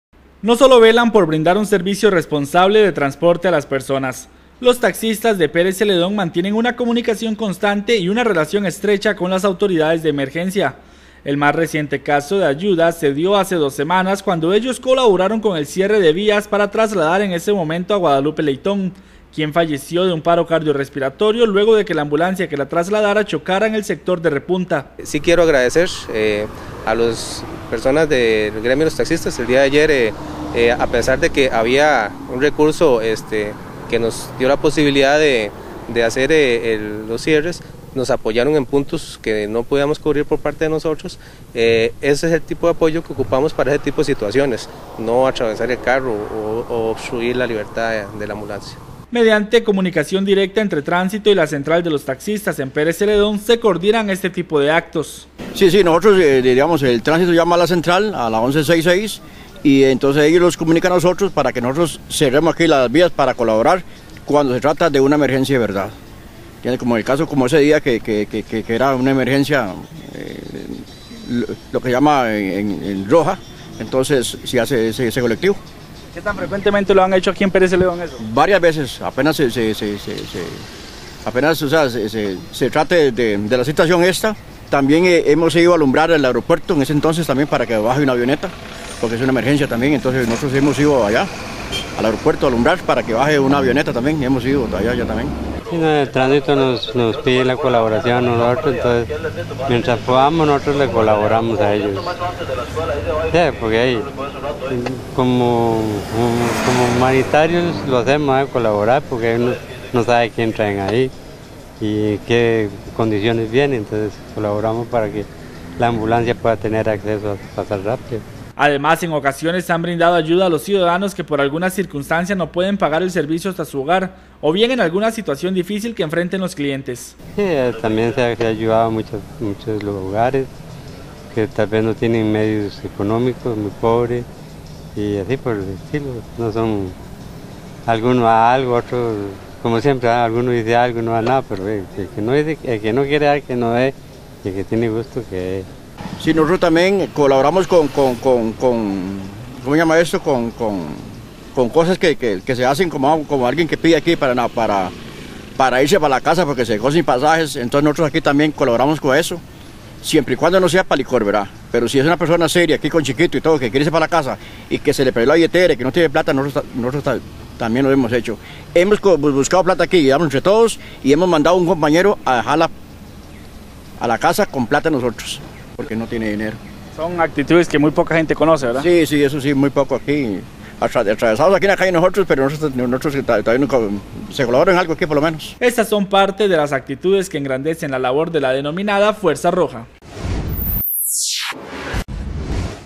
Los taxistas no solo transportan personas, también ayudan a los vehículos de emergencia
No solo velan por brindar un servicio responsable de transporte a las personas, los taxistas de Pérez Zeledón mantienen una comunicación constante y una relación estrecha con las autoridades de emergencia, el más reciente caso de ayuda se dio hace dos semanas cuando ellos colaboraron con el cierre de vías para trasladar en ese momento a Guadalupe Leitón, quien falleció de un paro cardio respiratorio luego de que la ambulancia que la trasladara chocara en el sector de repunta.